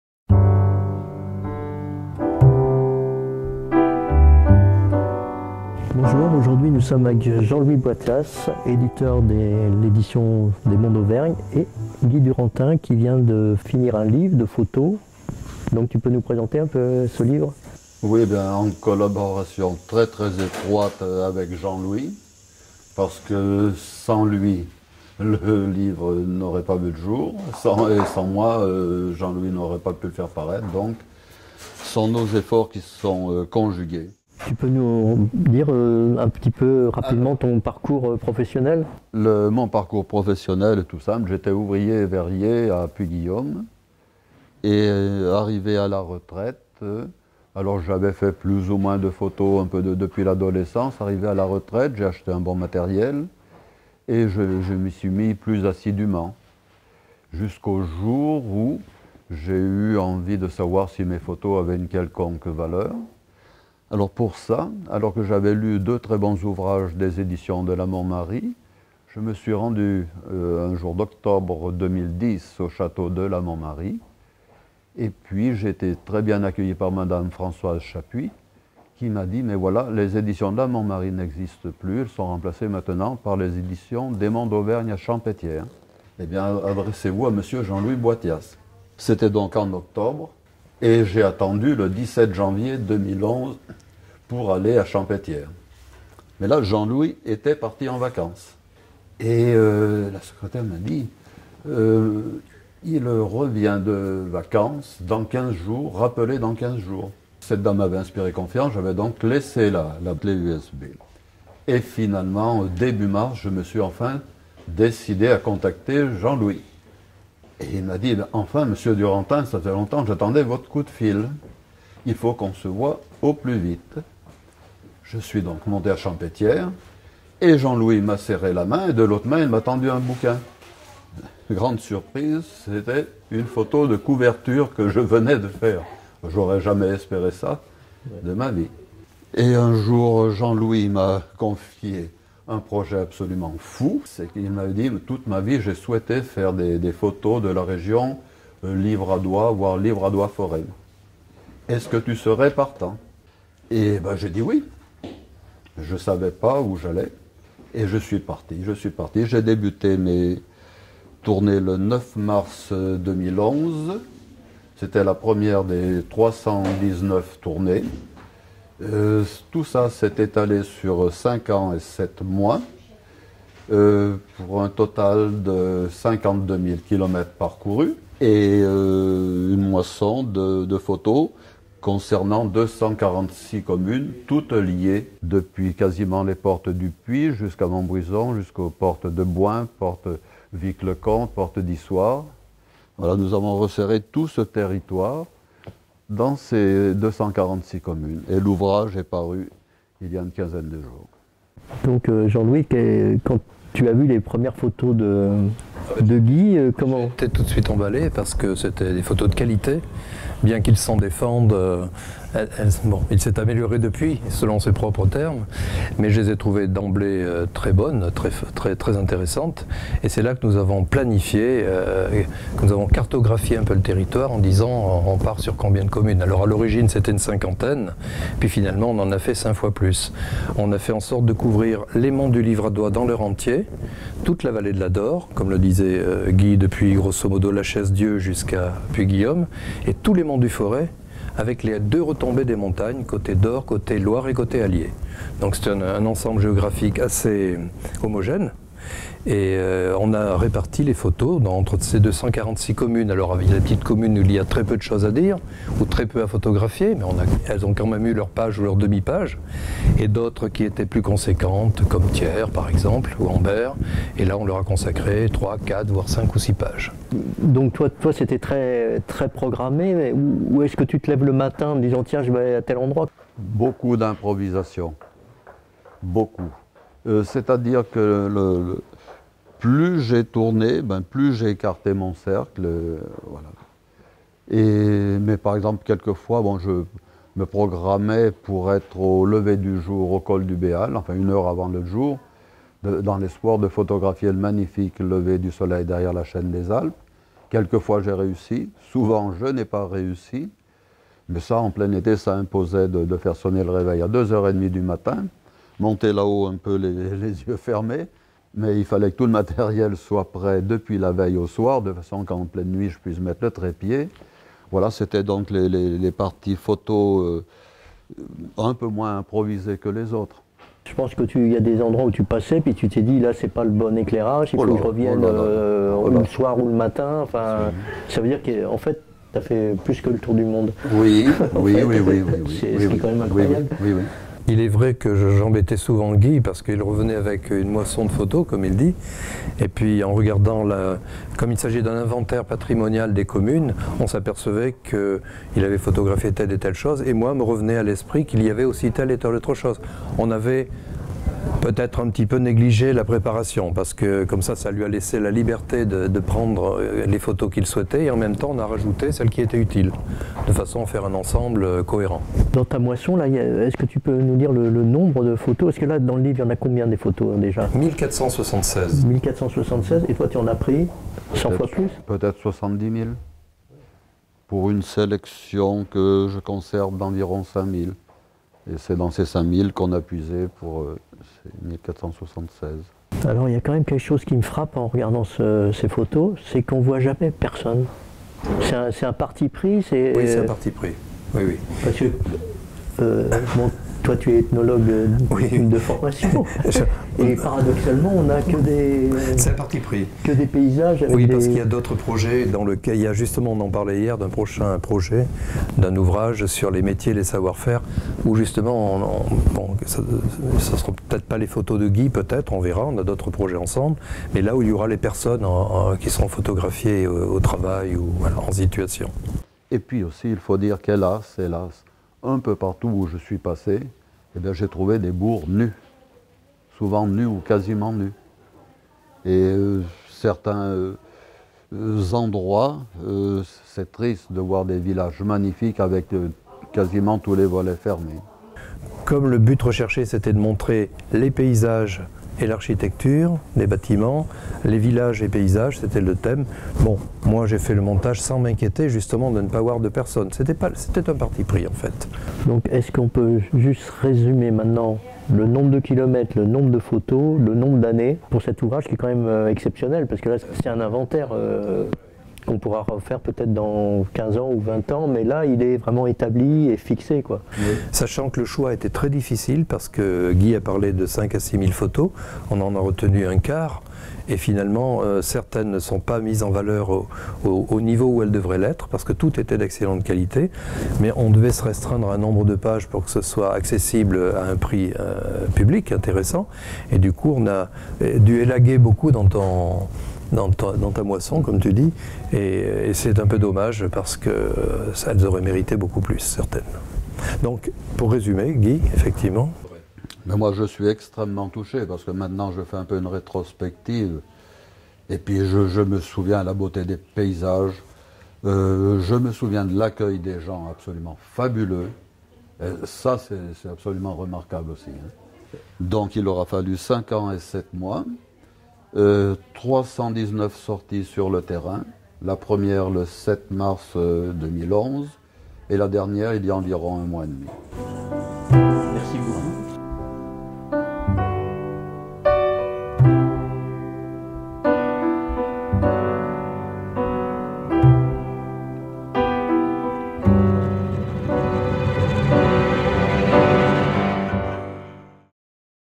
Balade photographique en Auvergne, Forez et Velay
Présentation d'un livre photographique de Guy Durantin édité par Jean-Louis Boithias des éditions des Monts d'Auvergne.. De l'Auvergne au Forez et au Velay. Une balade photographique au carrefour des trois provinces.